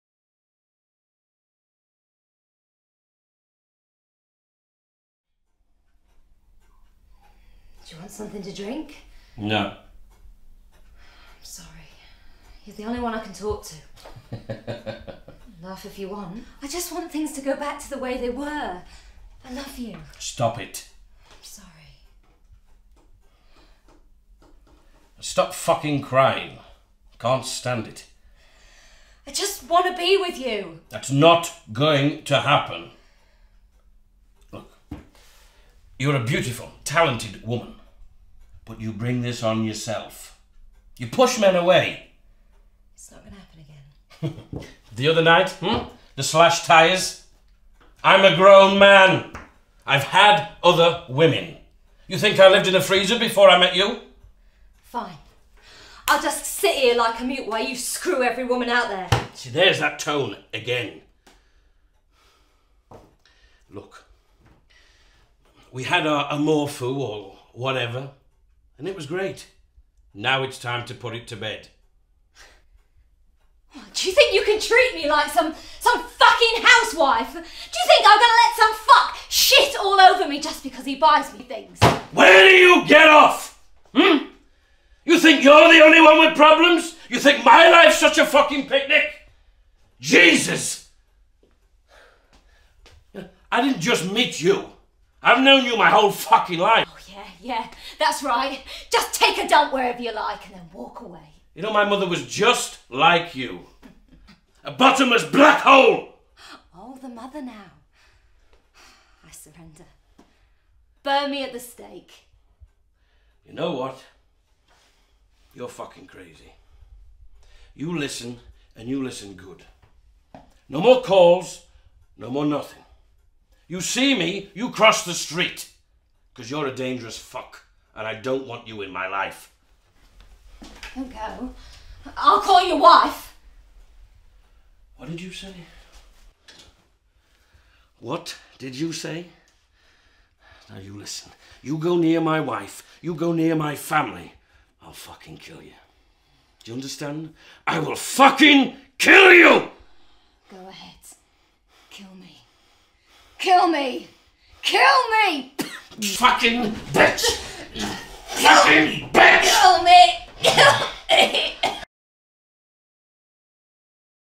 'Amour Fou' - David Chase - Excerpt from Steven K. Beattie showreel 2016
'Amour Fou' is adapted from a script for 'The Sopranos' by David Chase and this clip is taken from Steven K. Beattie's 2016 showreel.

Characters here are played by Steven K. Beattie and Shelley Anne Rivers.

Camera - Philip Kempson of C M Productions
Sound - Tim Vickerstaff

Thanks also go to Tim and Amanda owners of the filming location.